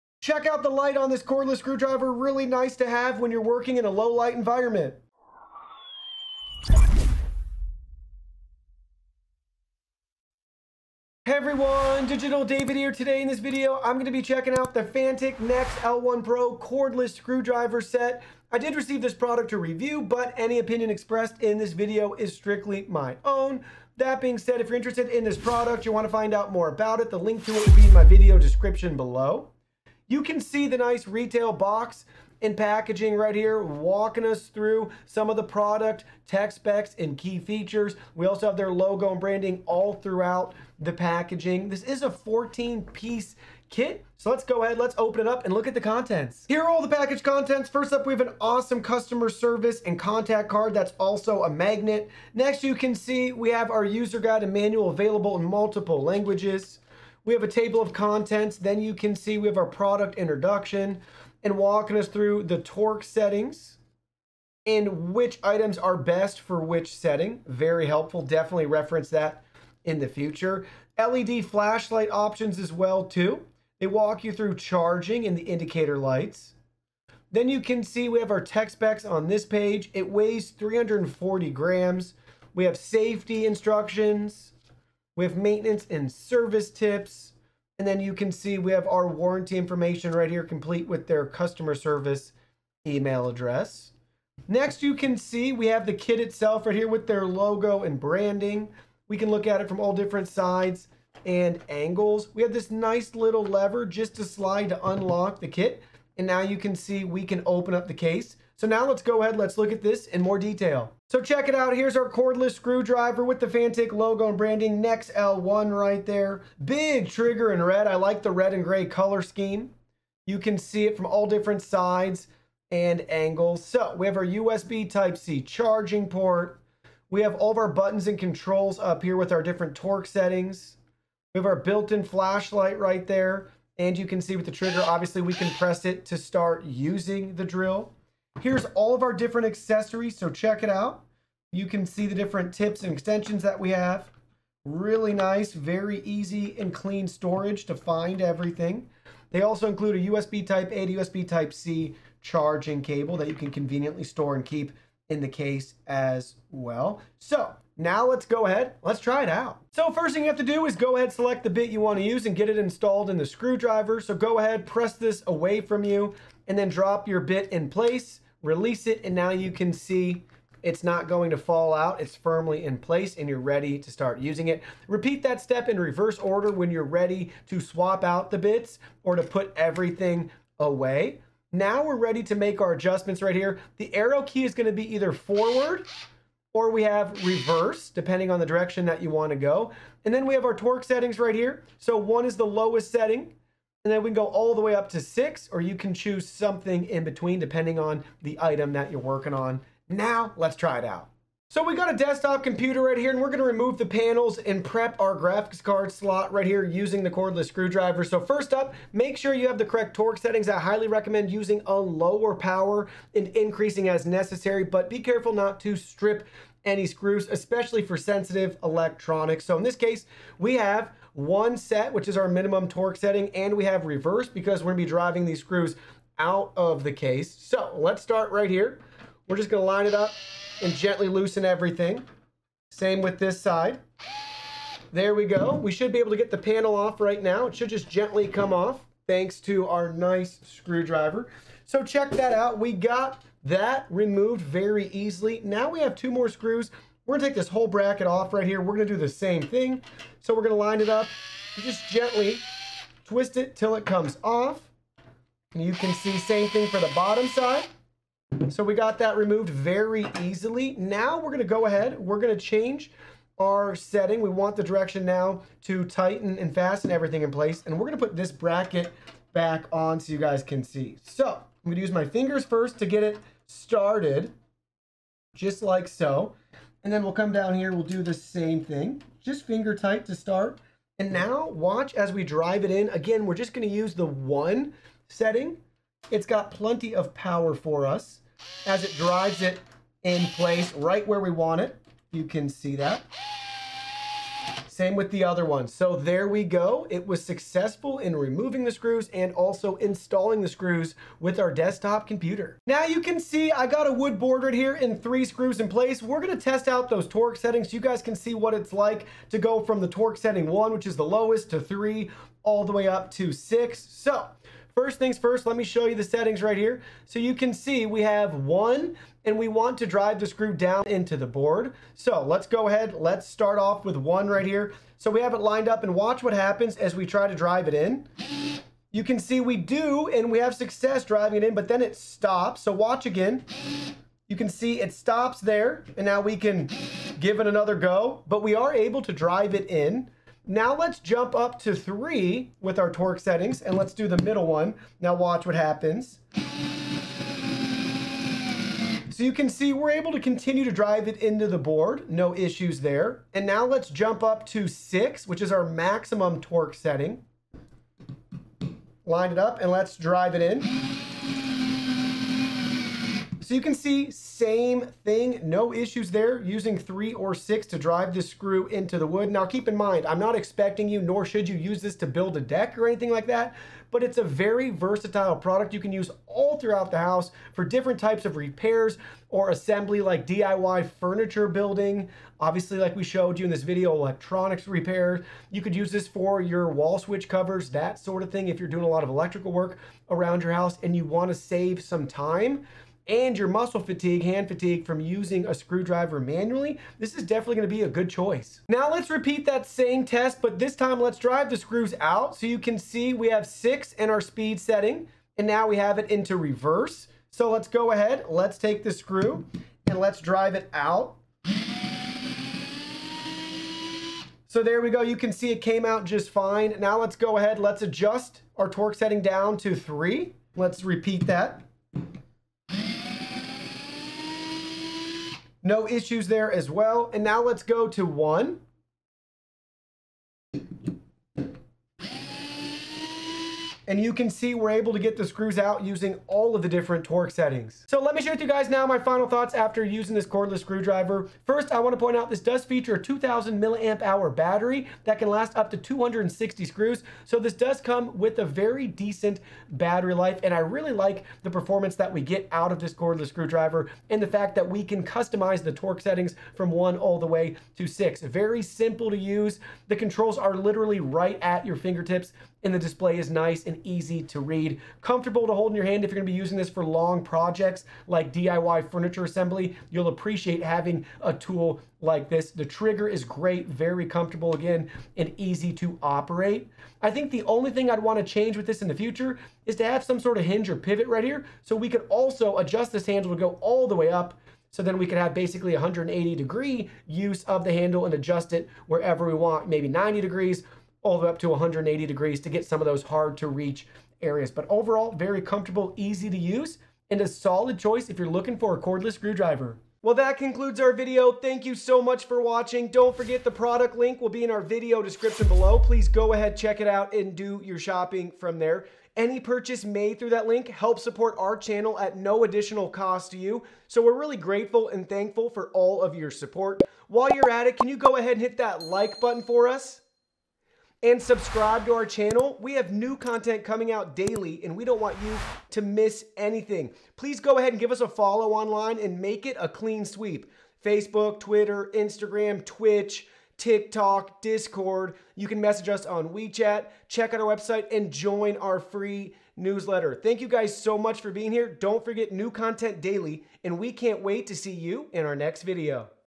Fanttik Cordless Screwdriver Kit // NEX L1 Pro Review
Fanttik Cordless Screwdriver Kit
Amazon INTL

Fanttik Cordless Screwdriver Kit 14pcs, Digital Screen, Li-ion 2000mAh, 6 Gears 0.5-6 Nm Rechargeable Electric Screwdriver Set 3.6V with USB Cable, Front LED, Yellow - NEX L1 Pro

00:00 - Intro
00:46 - Contents
01:50 - Close Up
02:39 - Setup
03:21 - Using It
05:02 - Torque Settings
06:59 - Final Thoughts

I received this product for free along w/ additional compensation but any opinion expressed is strictly my own!